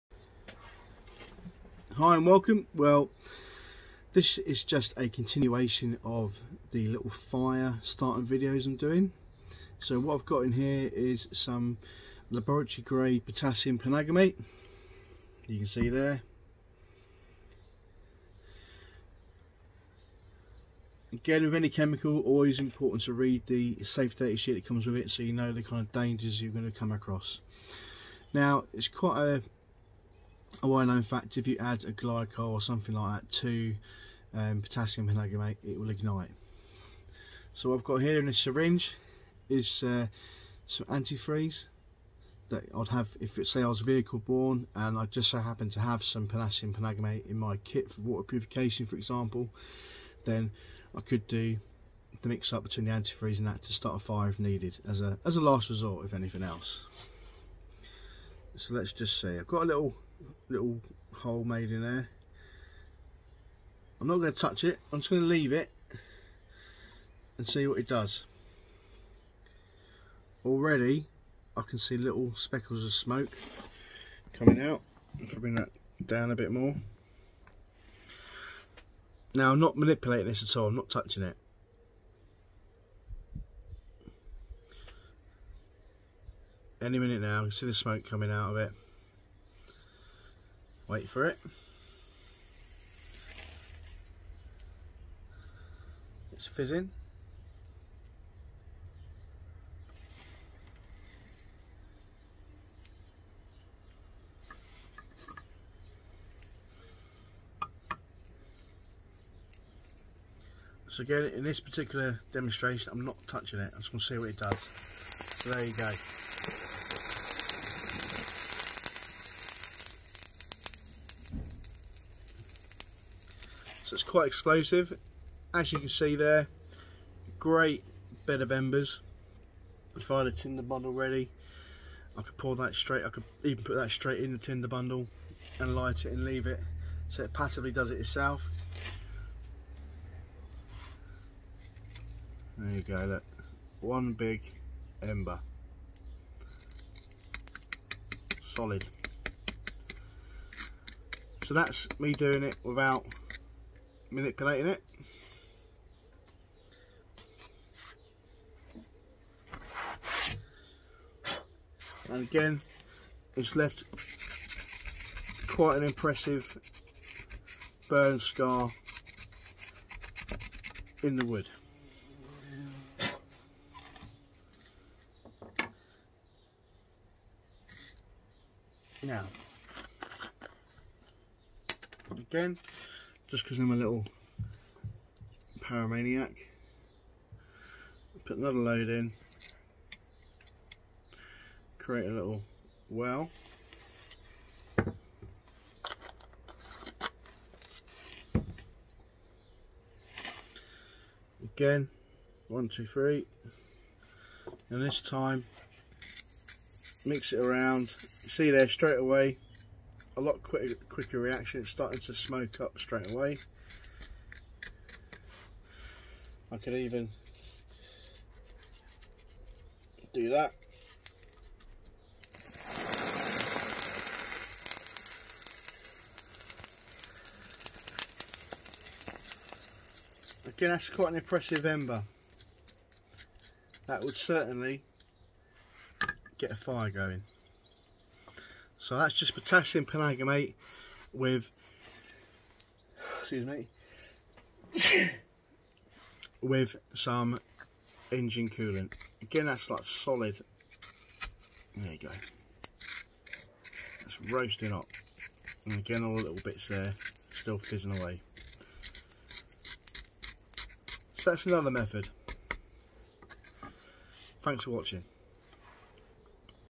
Starting a fire with potassium permanganate and antifreeze
In this short vid I am just practicing some fire starting skills using different techniques as it's good to have options.